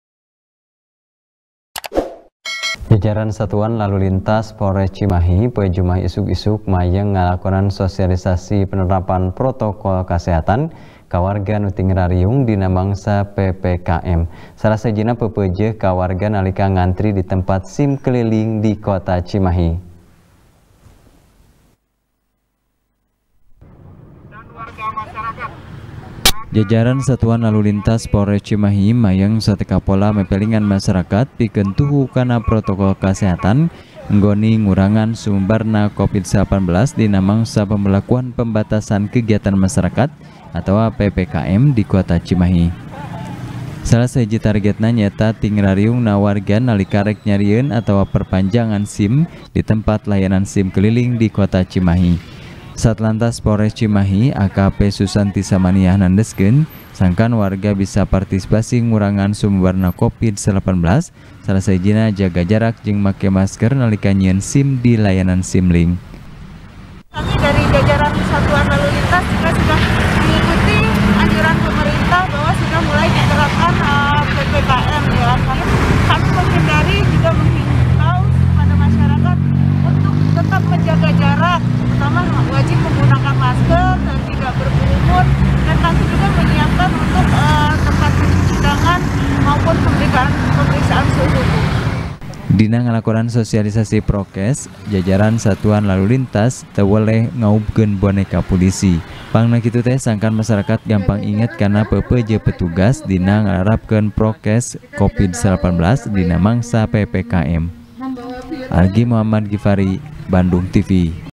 Prokes Dina Sim Keliling Mangsa PPKM Kota Cimahi | TP | BANDUNGTV
TP | BANDUNGTV | IWAN M RIDWAN | Jajaran Satuan Lalu Lintas Polres Ciimahi, Poe Jumaah Isuk-Isuk, Mayeng Ngalakonan Sosialisasi Penerapan Protokol Kesehatan, Ka Warga Anu Tingrariung, Dina Mangsa PPKM. Salah Sahijina Peupeujeuh Ka Warga Nalika Ngantri Di Tempat Sim Keliling Di Kota Cimahi.